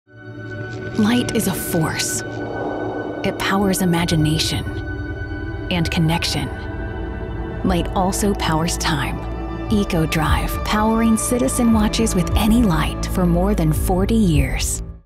Citizen Purposeful Power Brand Commercial - Pepsi Dive
Citizen Watch with Eco-Drive, the sustainable way to power your watch with light, any light. Invented over 45 years ago, Citizen’s proprietary Eco-Drive is the light-powered, eco-mindful technology that eliminates the need for watch battery replacement. It’s power with a purpose.